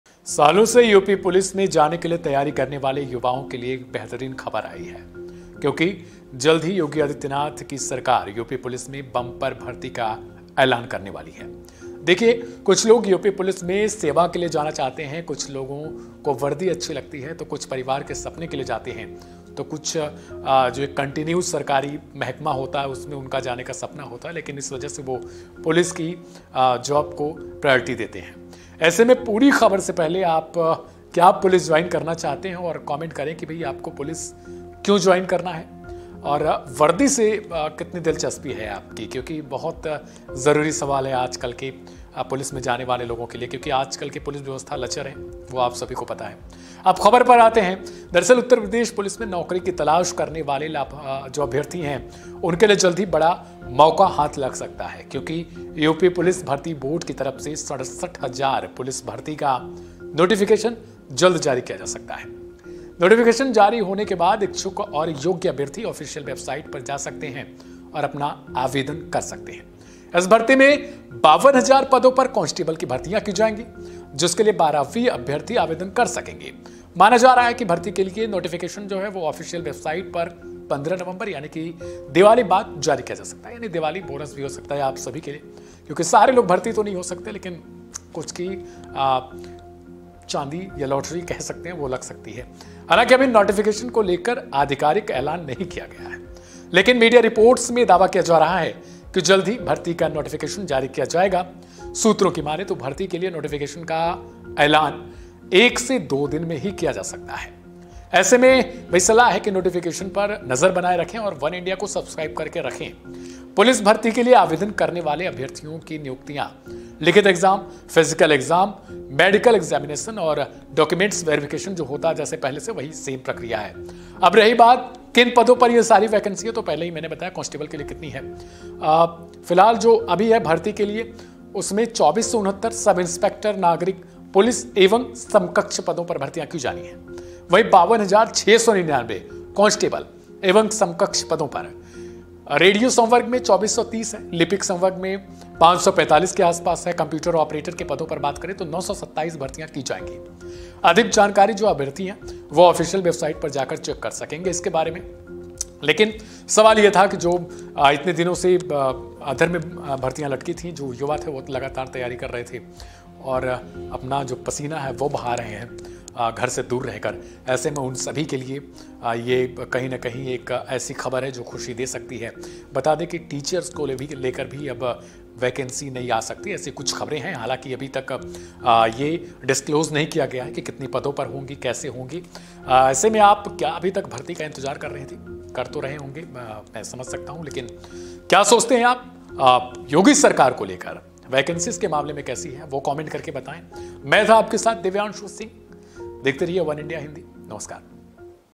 CM Yogi Adityanath की सरकार UP Police में जल्द लाएगी नई भर्ती | New Government Job | वनइंडिया हिंदी
UP Police Bharti 2023: दिल्ली (Delhi) बाद यूपी पुलिस (UP Police ) में नौकरी की तलाश करने वाले अभ्यर्थियों को बड़ा गिफ्ट मिल सकता है. मीडिया रिपोर्ट्स की मानें तो यूपी पुलिस भर्ती के लिए नोटिफिकेशन ऑफिशियल वेबसाइट पर 15 नवंबर को जारी किया जा सकता है. नोटिफिकेशन (Notification) जारी होने के बाद अभ्यर्थी ऑफिशियल वेबसाइट ( Official Website UP Police Recruitment & Promotion Board ) पर जाकर आवेदन कर सकेंगे.